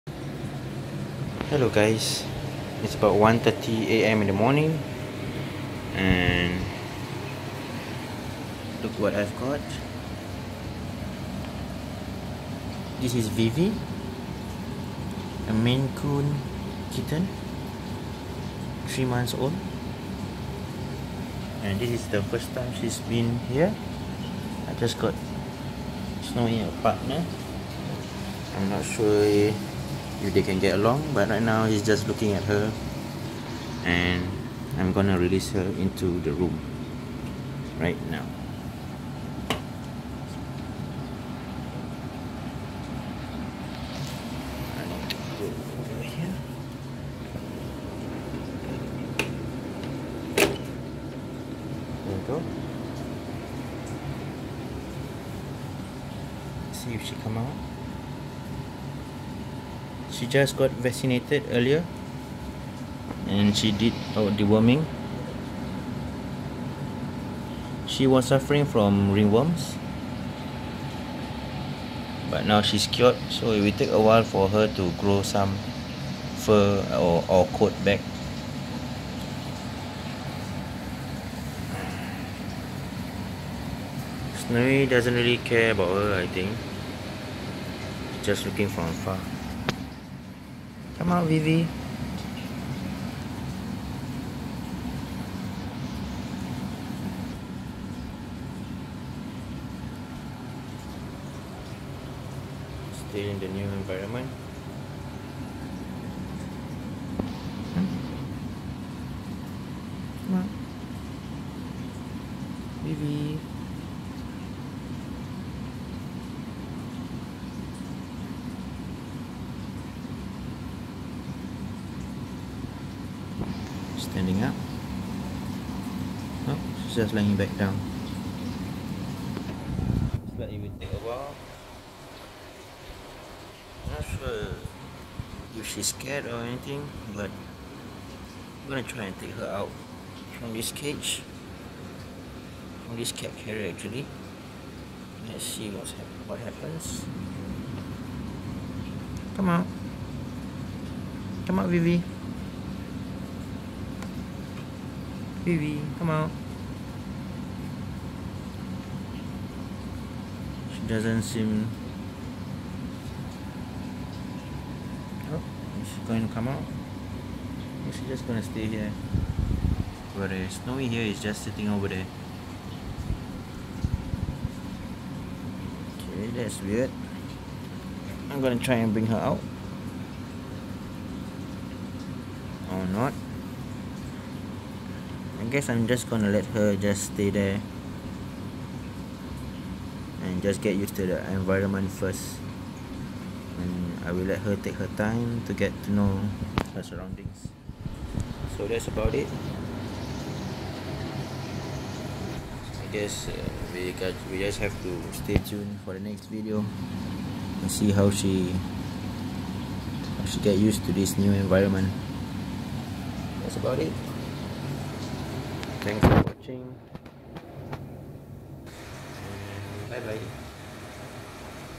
Ragdoll & Maine Coon Cat Vlog #11 (21 July 2018)
Just got a female Maine Coon kitten. She is currently 3 months old and her name is Vivi.

(Snowy Birthdate: 25 July 2017) Ragdoll male kitten.

(Vivi Birthdate: 07 April 2018) Maine Coon female kitten.